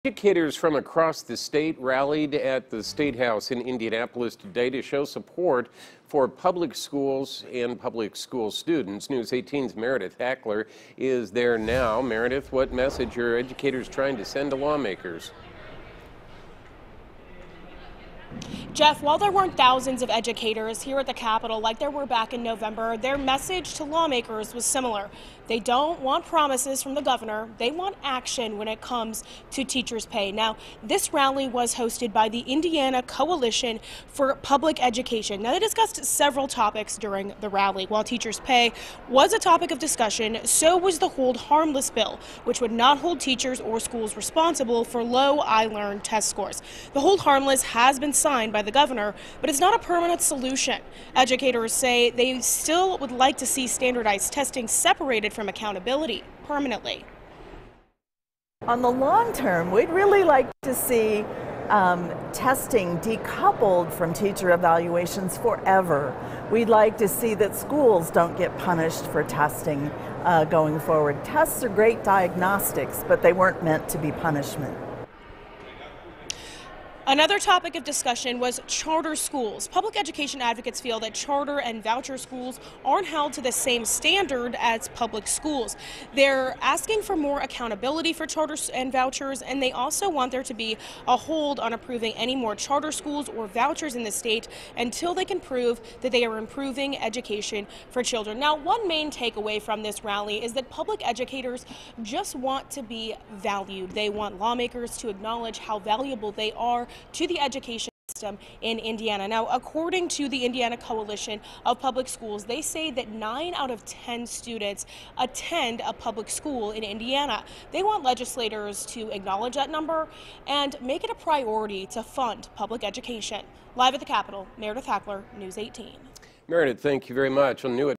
Educators rally Monday at statehouse for lawmaker action
Educators from across the state are rallying at the statehouse to show support for Indiana public schools and students.